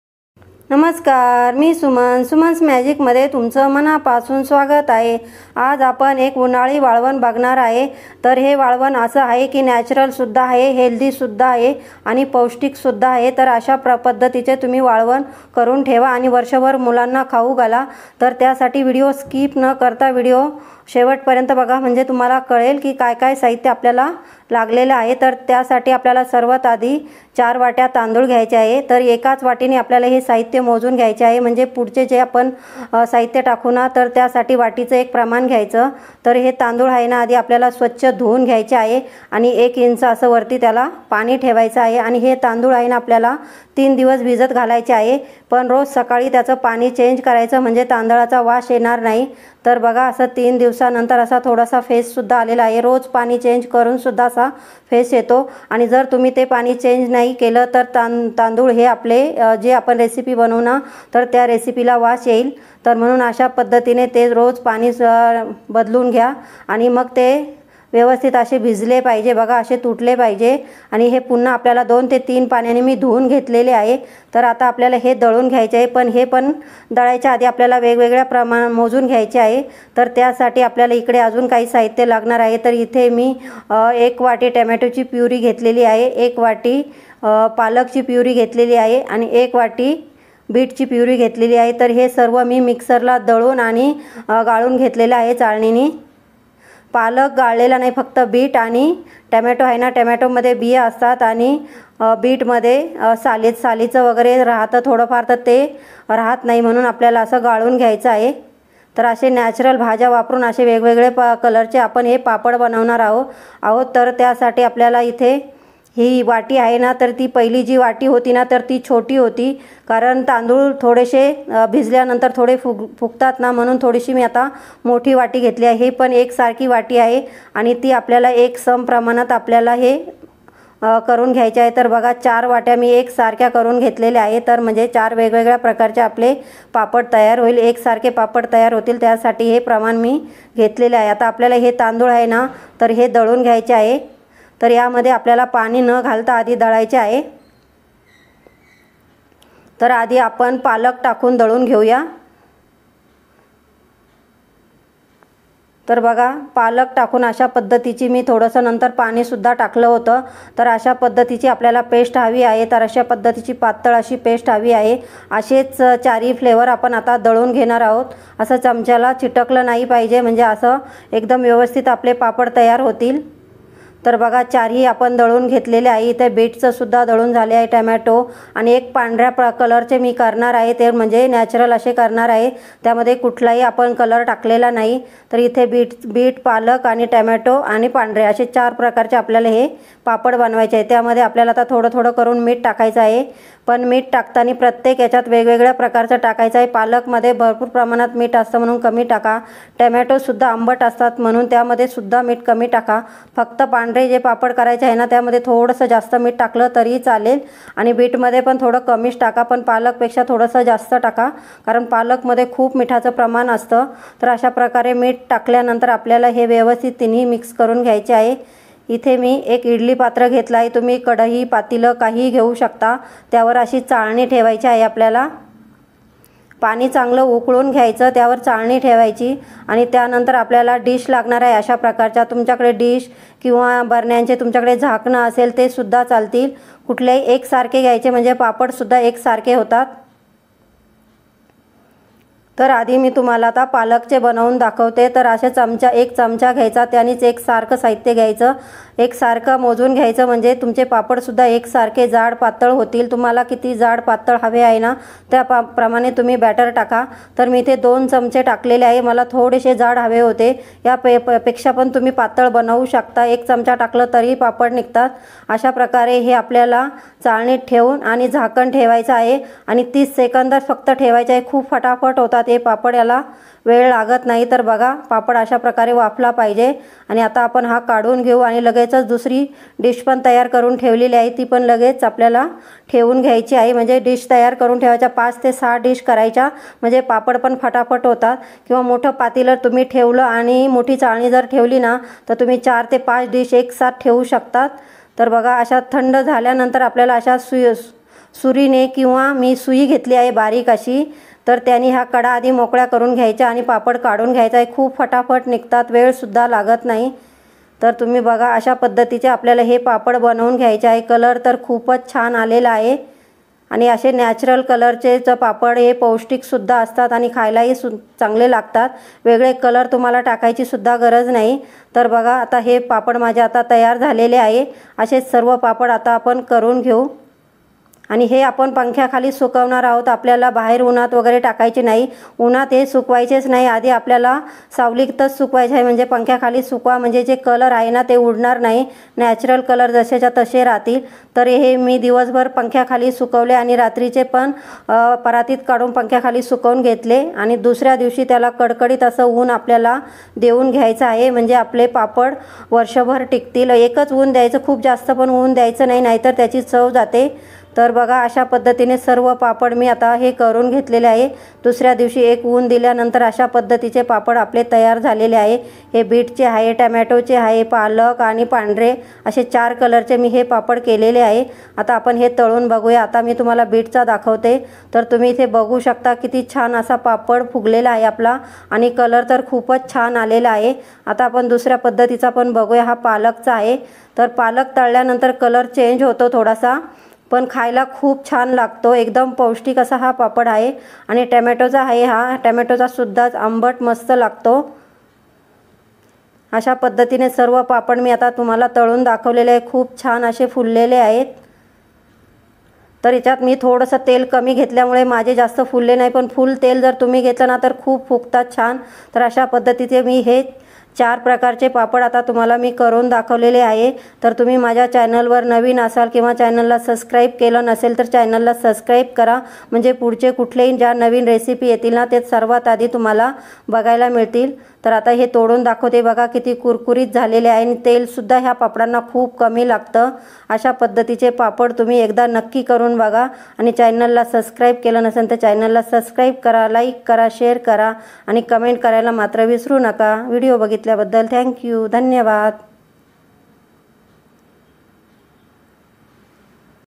उन्हाळी वाळवण /चार प्रकारचे तांदळाचे वाफेवरचे पापड /नॅचरल रेसिपी /Summar Recipe /Suman's Magic
: साहित्य :

1 वाटी पालक ज्युस / 1 वाटी बिटचा ज्युस /1 वाटी टोमॅटो ज्युस / मिठ